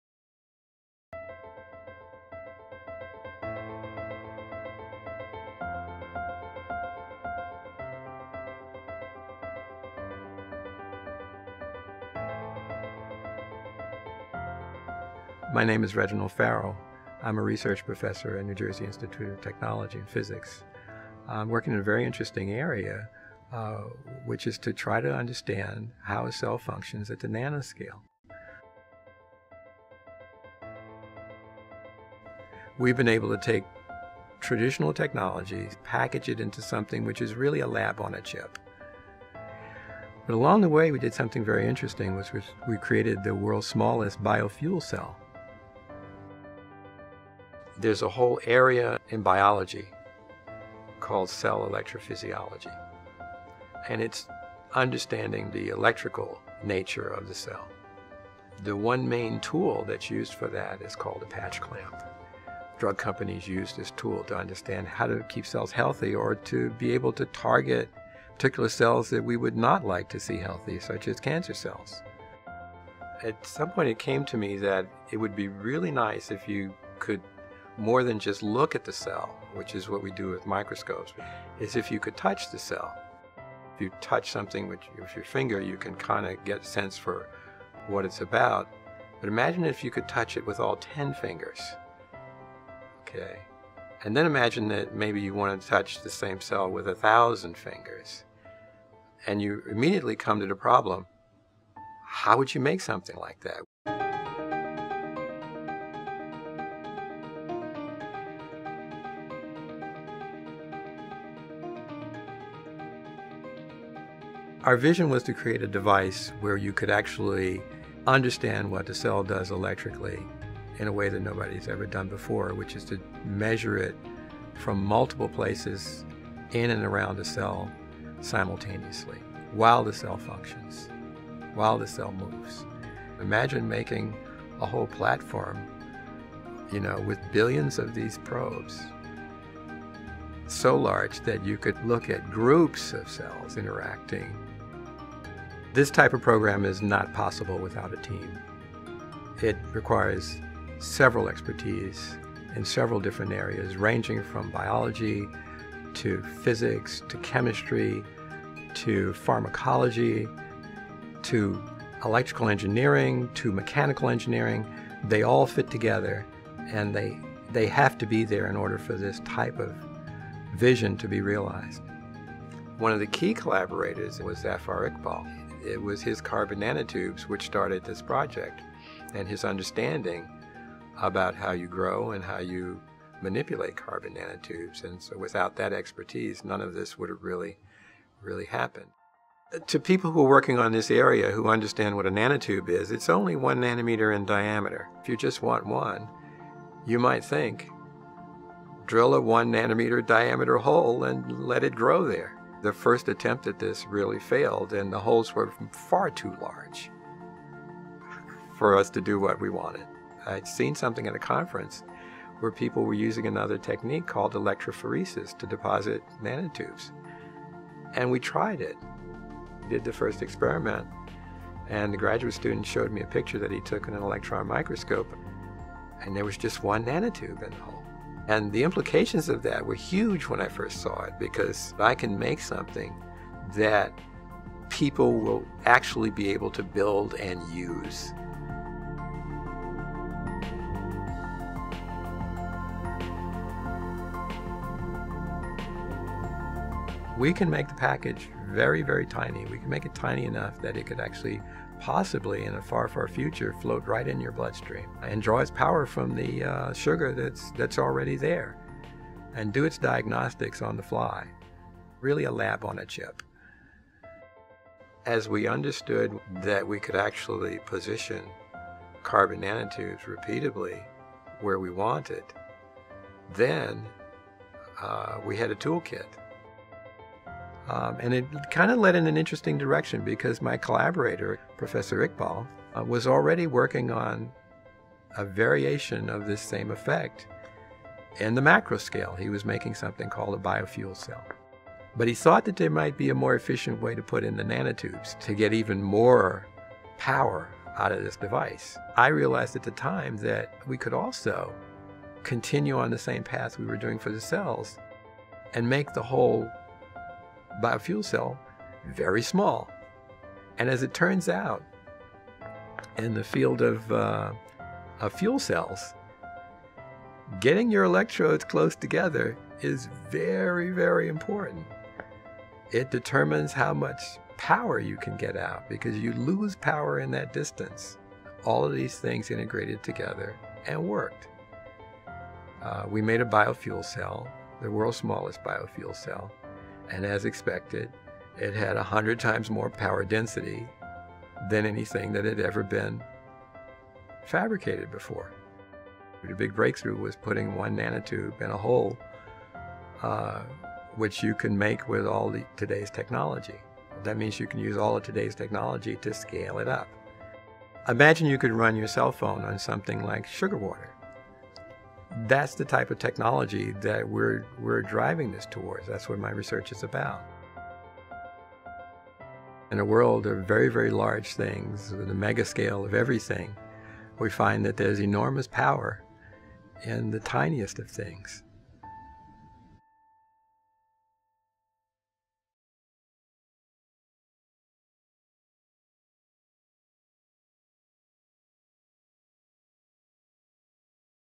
Reginald C. Farrow - NJIT Board of Overseers Excellence in Research Prize and Medal Awardee
The NJIT Board of Overseers Excellence in Research Prize and Medal is awarded to biophysics and materials science expert in nanotechnology Reginald C. Farrow.

Farrow and his team have used nanotube array platforms to investigate biological cells at the nanoscale. This cutting-edge work has already had a key role in advancing the development of a unique biofuel cell, engineering that could power in vivo biomedical devices using the body's own glucose and oxygen.